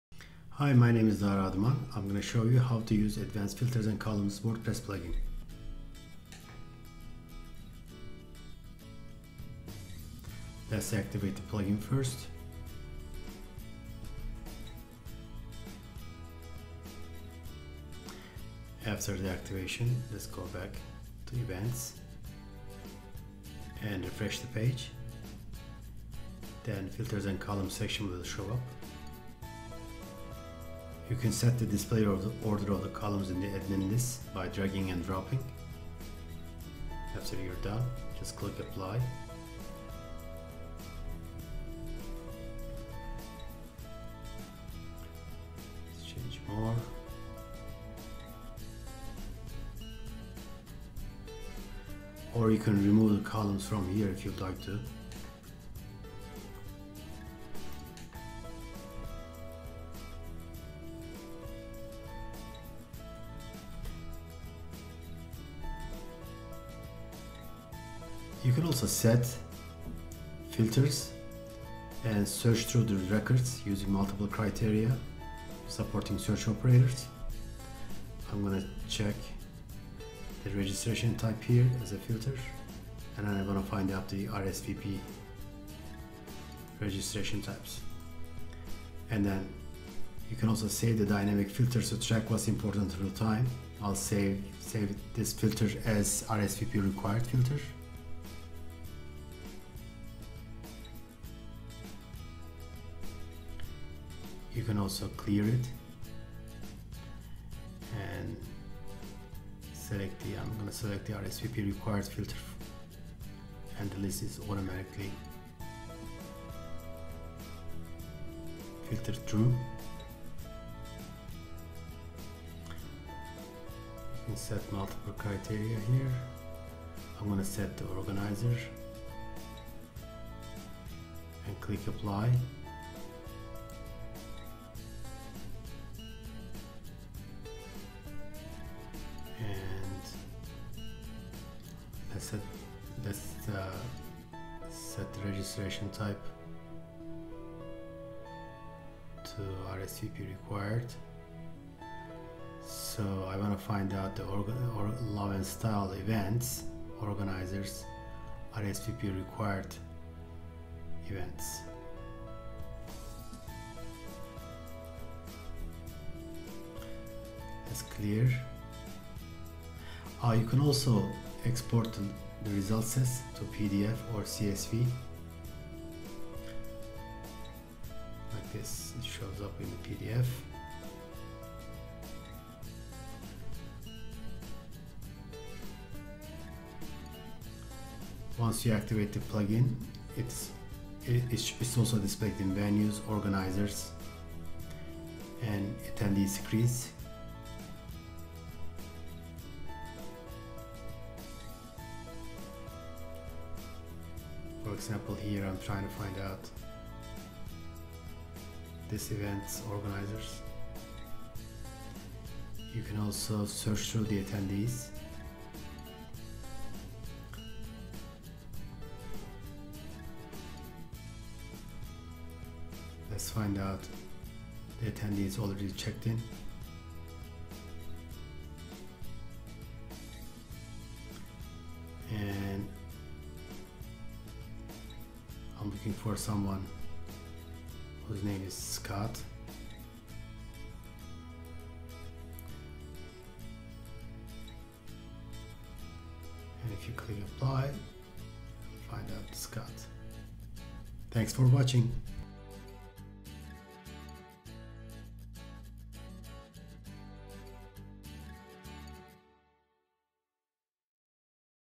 WP Easy Events WordPress Plugin - Advanced Filters and Columns Extension
WP Easy Events WordPress Plugin's Advanced Filters and Columns Extension allows to:
* set the display order of columns in the admin lists by drag and drop
* search using multiple criteria supporting search operators
* save dynamic filters to track what's important through time
Extension is enabled for events, venues, organizers and attendees.